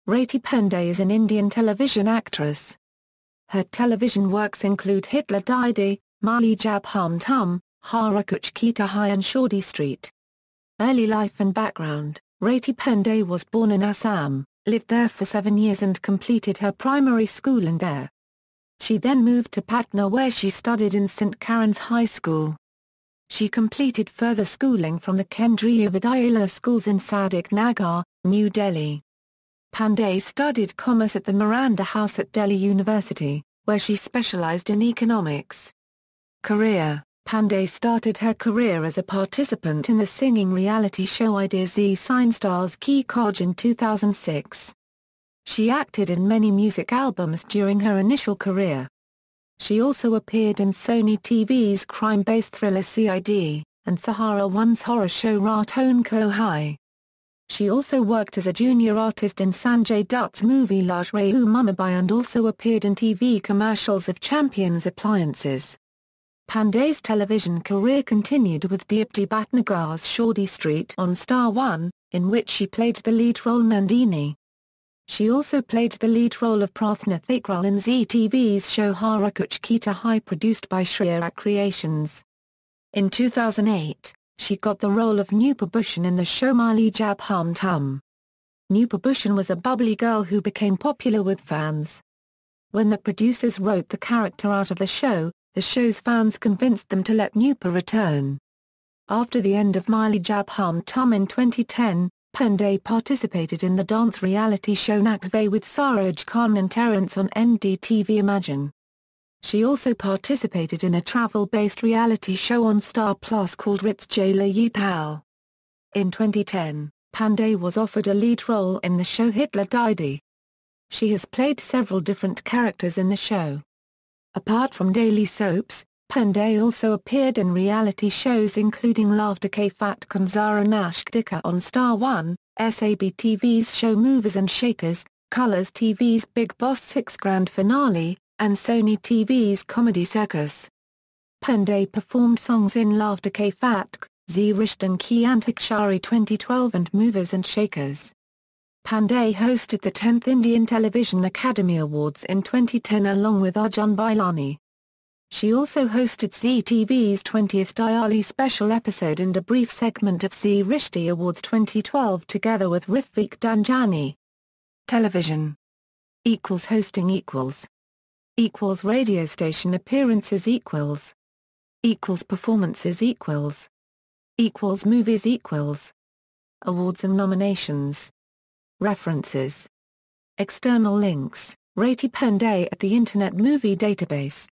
Rati Pandey
Rati Pandey is an Indian television actress. Her television works include Hitler Didi, Miley Jab Hum Tum, Har Ghar Kuch Kehta Hai and Shaadi Street.